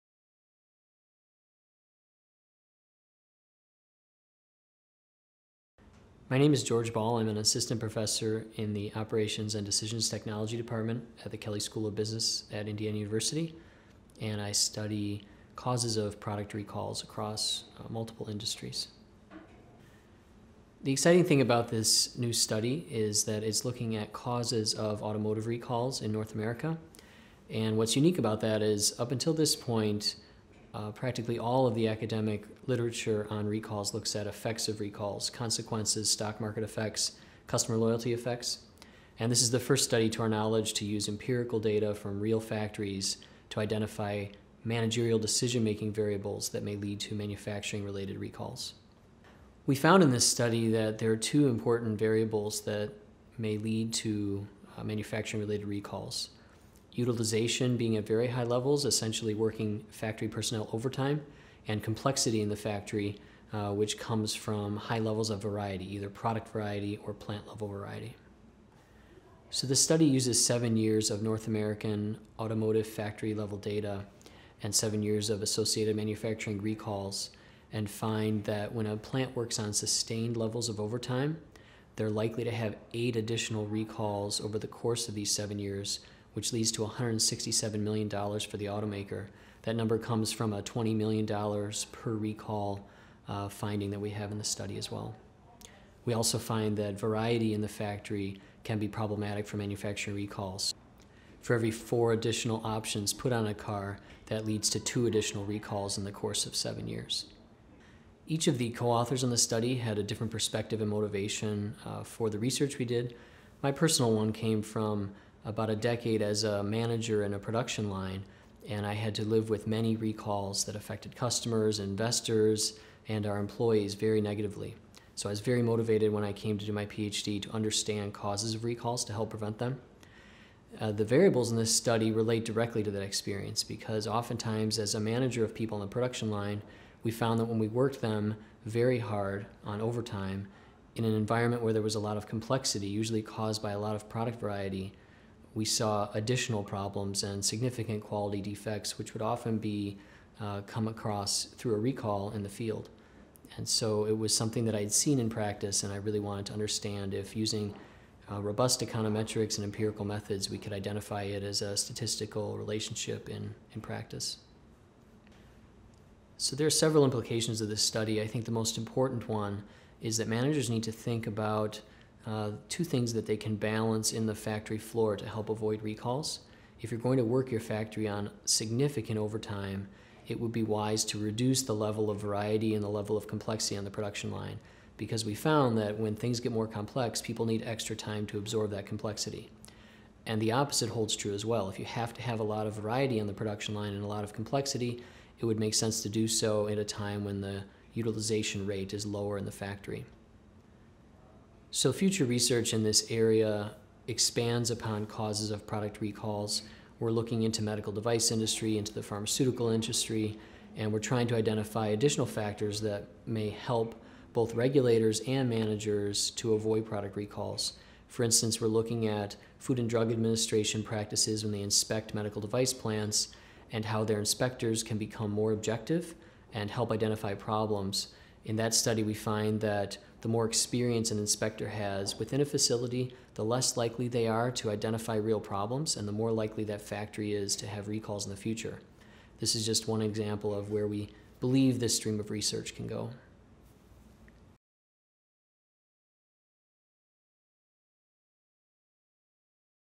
Overtime and adding options led to auto recalls costing $167 million
A study of North American auto production over a seven-year period finds that using overtime and increasing the number of factory-installed options are positively associated with manufacturing-related recalls. The study found that the economic cost of extremely high factory utilization -- operating near or at overtime levels -- translates into a recall cost of $167 million over seven years. George Ball, assistant professor of operations and decision technologies at IU’s Kelley School of Business and one of the study co-authors, explains his findings.

Video by Cara Swakopf for the IU Newsroom.